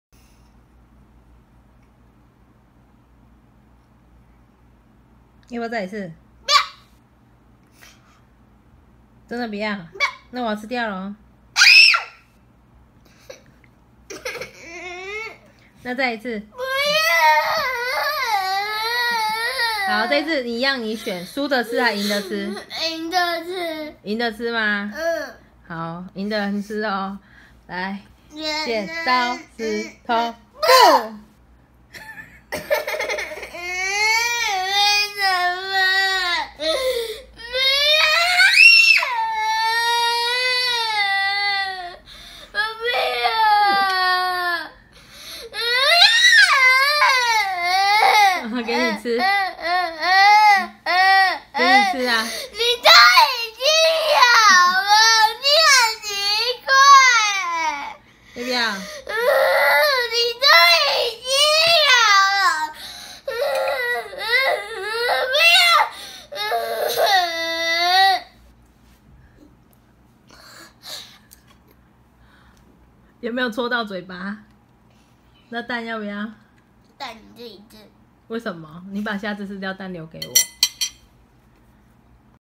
為什麼我都吃不到
吃不到就森七七欸😂
明明自己猜拳輸了還不服輸，上一秒說不要身體卻很誠實😄
嘴巴開這麼大，到底是有多好吃，就怕以後再也吃不到了😆😆😆